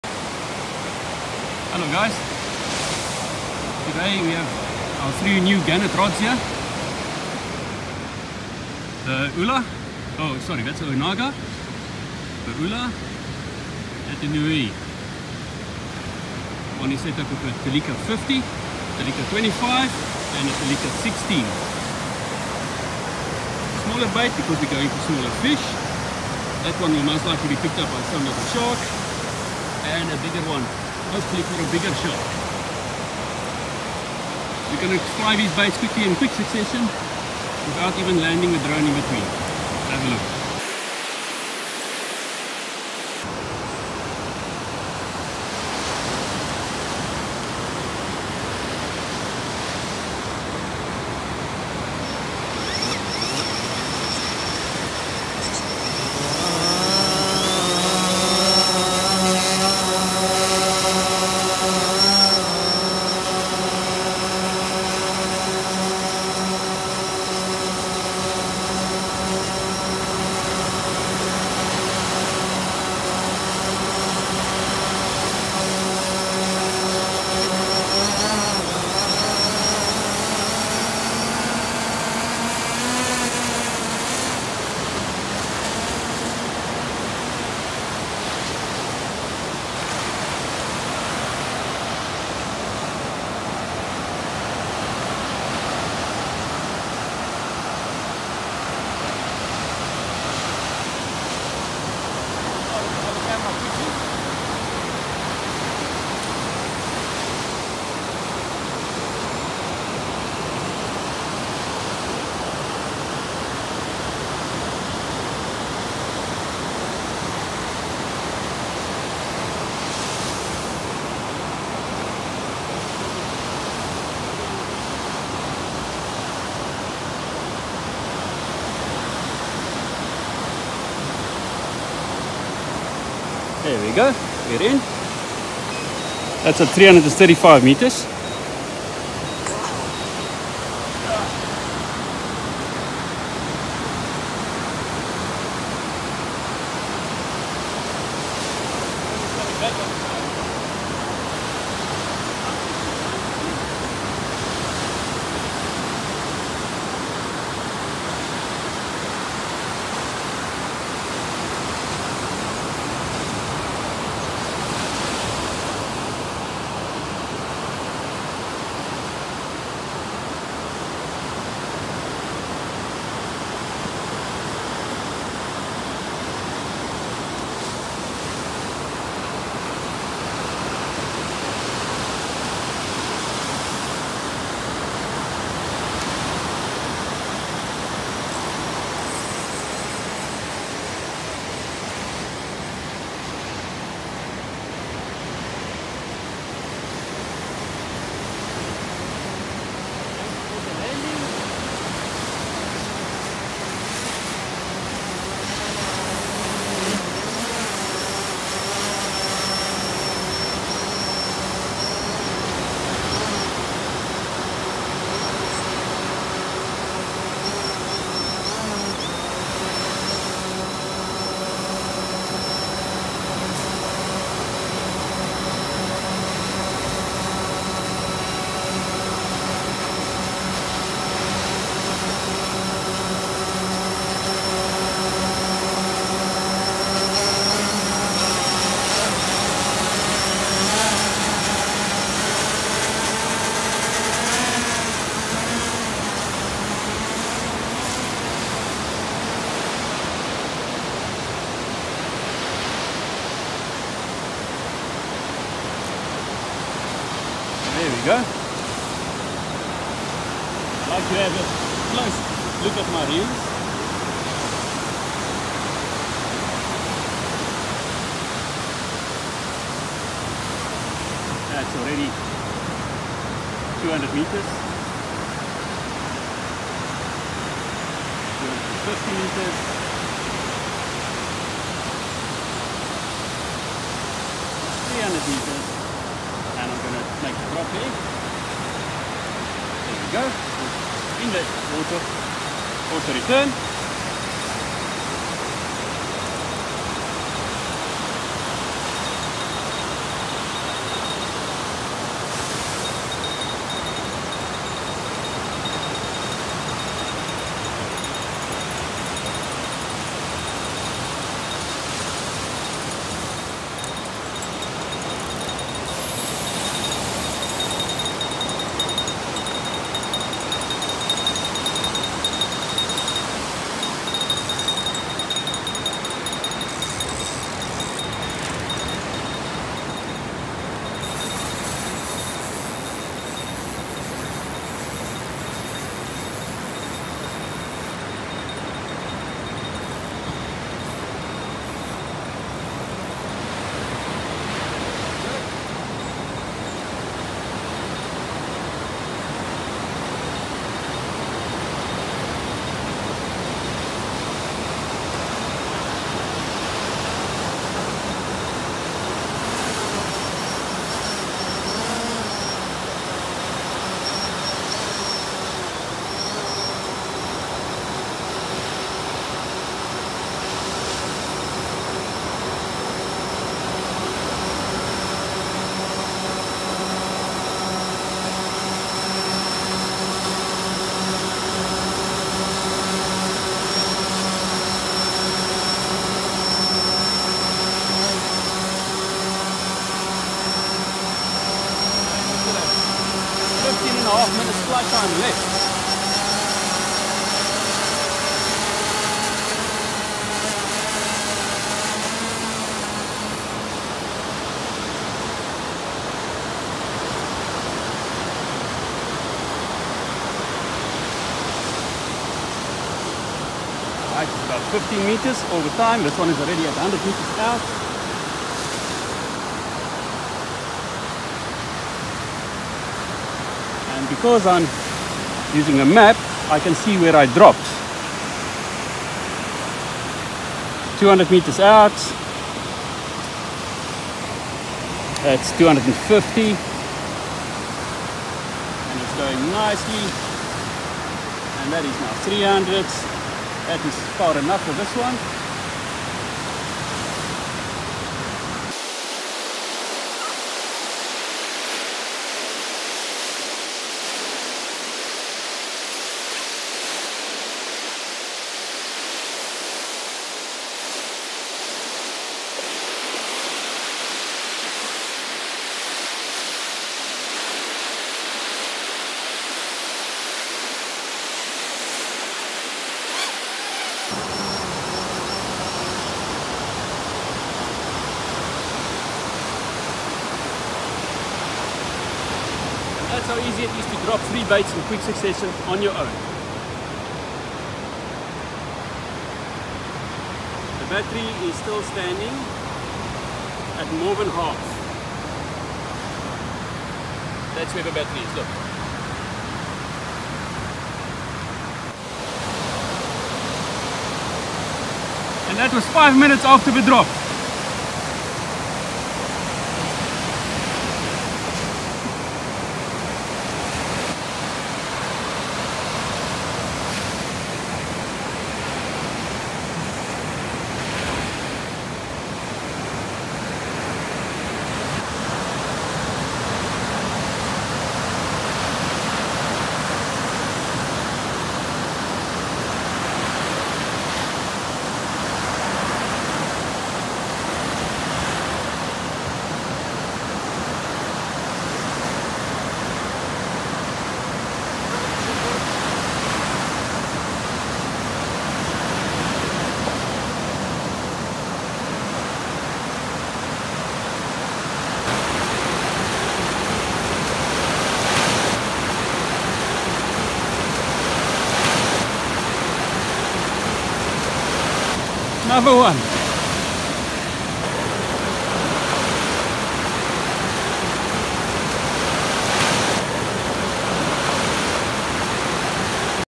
Gannet drone fishing with a DJI Phantom
Drone fishing made simple with a DJI drone fitted with a Gannet bait release.
Using the right equipment from hooks to rods and reels can make drone fishing effortless.